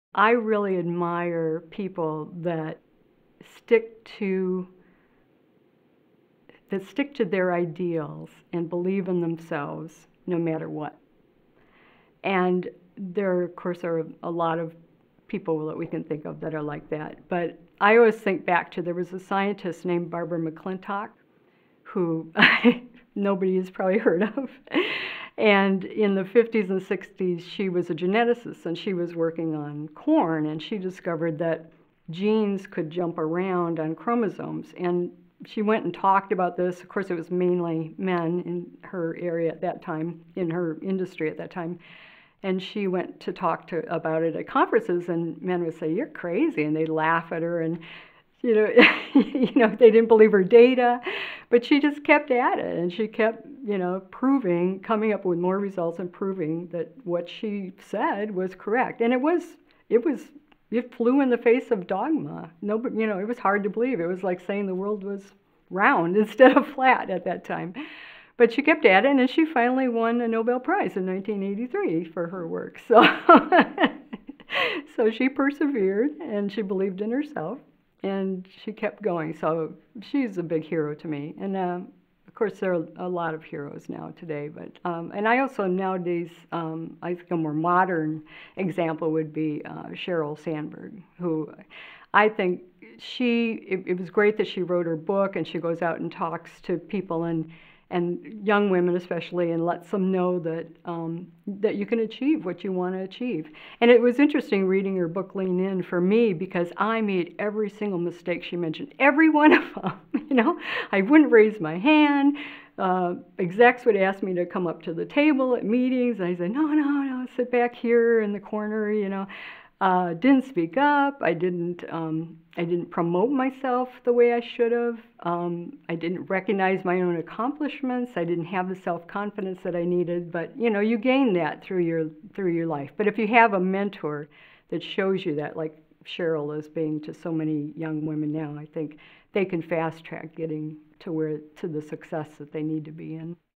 Molecular Biologist: Heroes - Sylvia Norman Career Girls Role Model
Molecular biologist Sylvia Norman, Ph.D., shares valuable career guidance and life advice with girls.